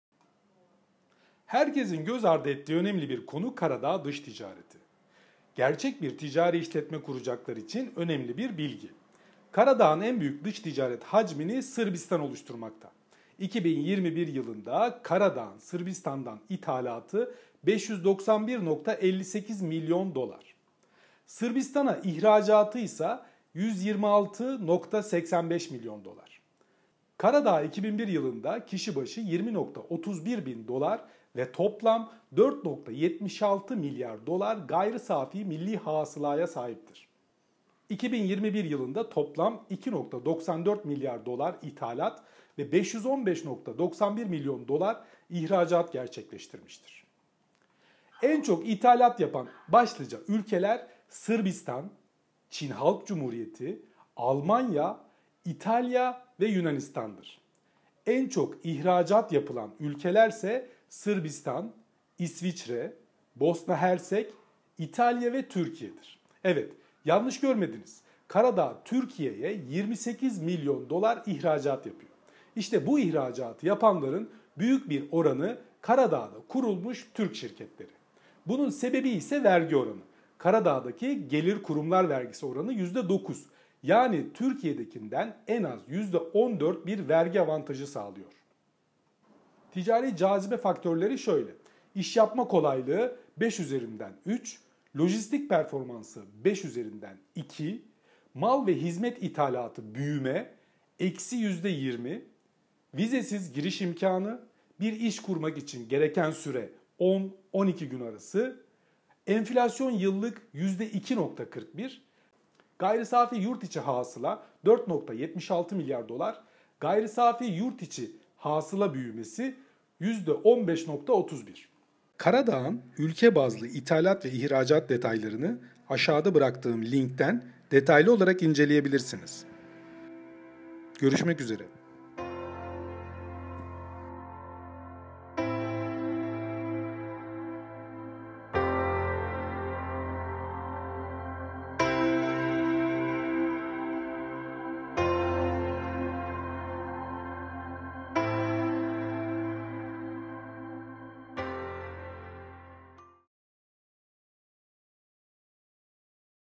Karadağ Dış Ticaret Bilgileri Yıllık İhracat ve İthalat
Karadağ dış ticareti ile ilgili gerekli bilgileri sizinle paylaştık.
Yeni şirket kuracak ve ticaret yapacaklar için umarım en azından küçük de olsa bir bilgi olur.